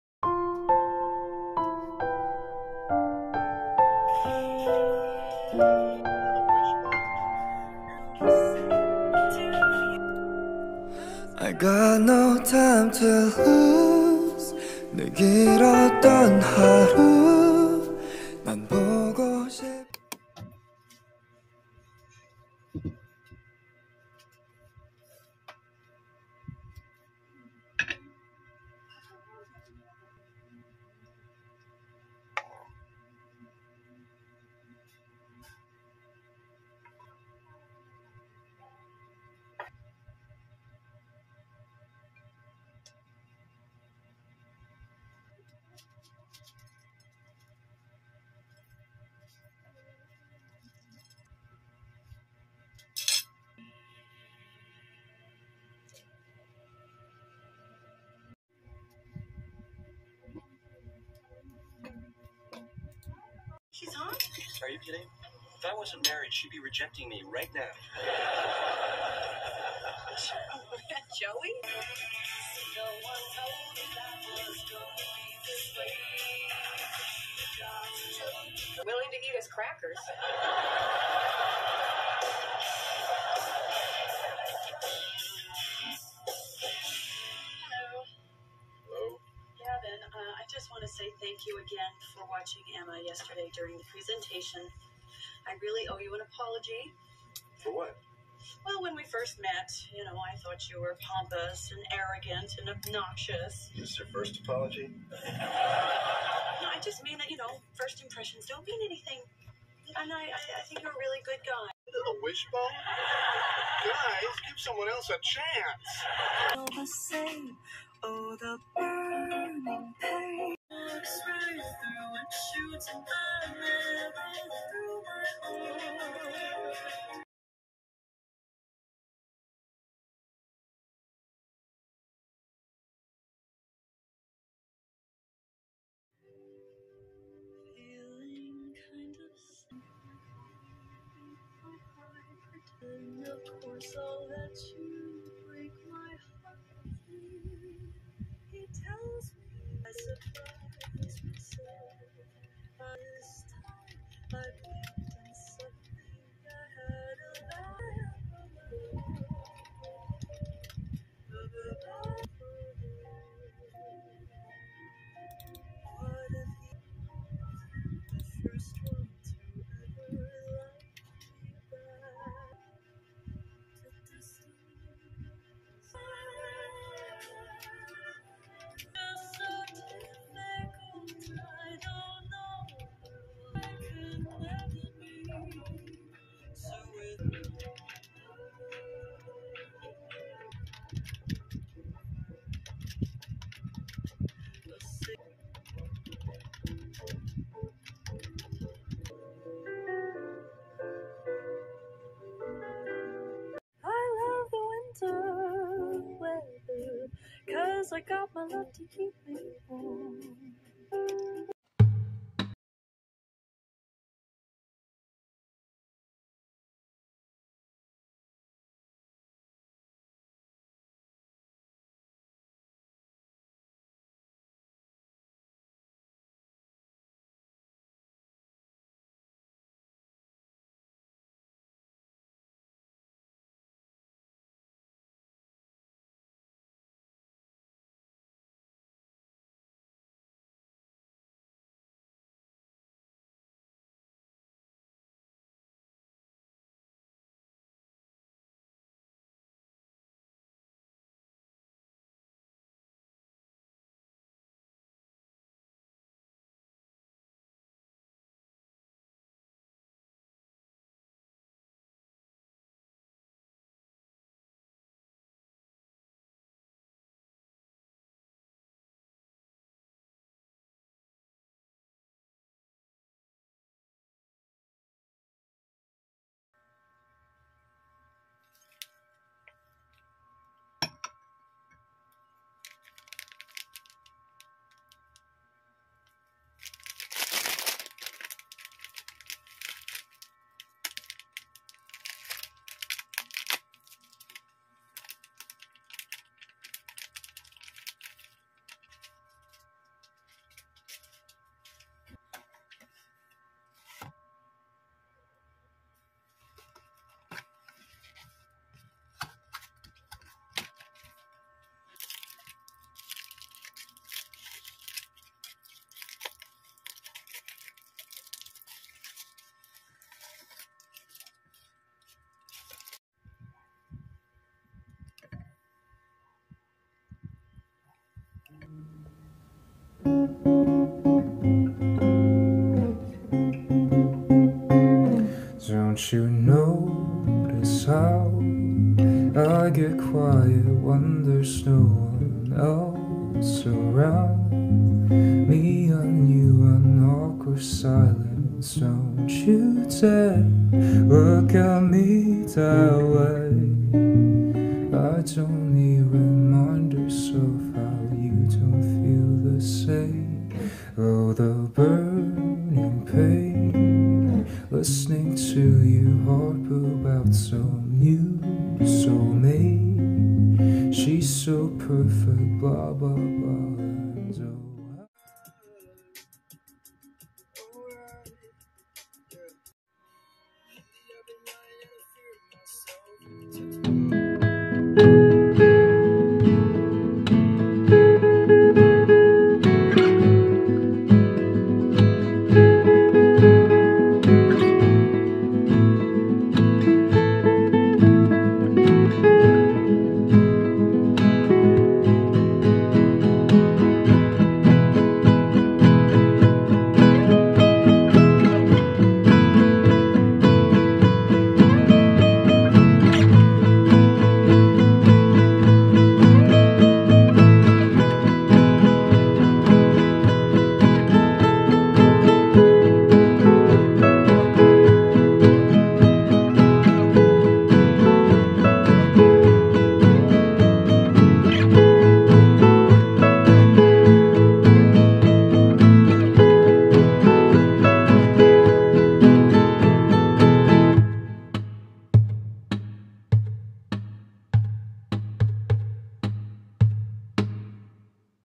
night study vlog 🌜 | revising my essay, keyboard asmr, cramming my poster, hot choco 🍫| daily vlog☘️
Doing study sessions at night, with yellow lamp on and a slow music, makes it somehow so peaceful :) I hope you had fun watching the vlog ! Thank you for your support 💛
Usual Upload Schedule: sun/mon

music ♪⁠～ !! (please support them!)